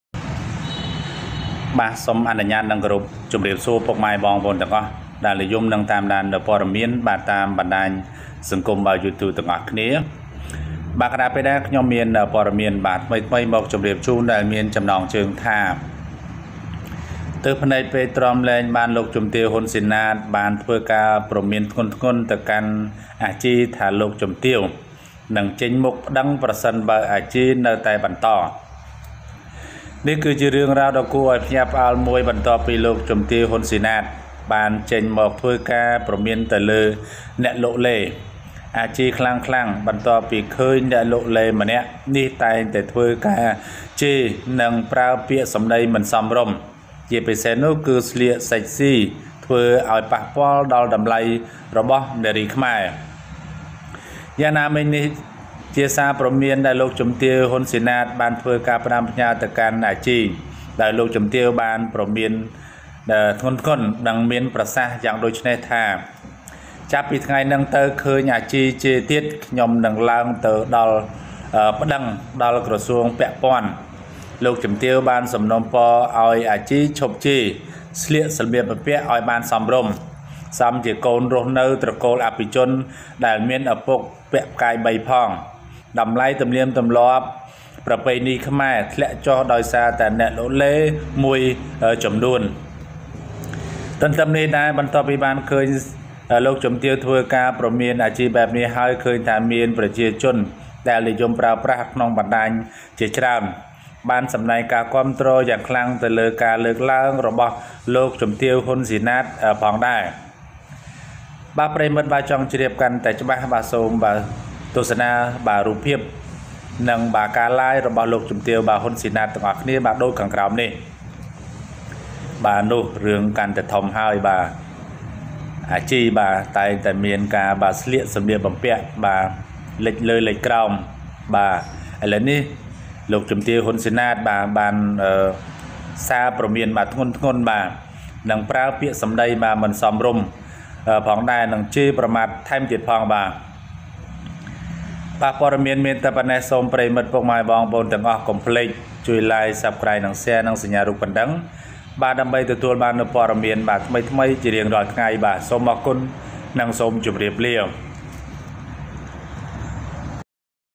Too big eyes! Unable to resist, Lok Chumteav Hun Sinath issued a stern warning to Aji that she wo
Too big eyes!  Unable to resist, Lok Chumteav Hun Sinath issued a stern warning to Aji that she would file a lawsuit if Aji continued ... (Video)

ទើសភ្នែកពេក ! ទ្រាំលែងបានលោកជំទាវ ហ៊ុន សុីណាត បានធ្វើការព្រមានធ្ងន់ៗទៅកាន់ អាជី ថាលោកជំទាវនិងចេញមុខប្តឹង ប្រសិនអាជីនៅតែបន្ត...(មានវិដេអូ)